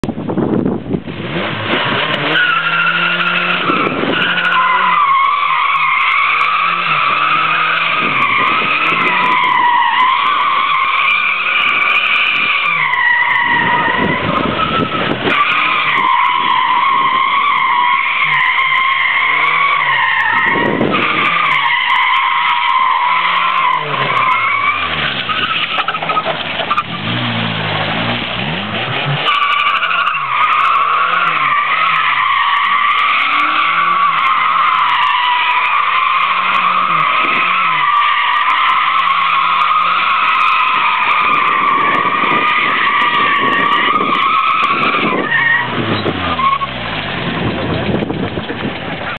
240sx welded diff donut
me just burning up sum tires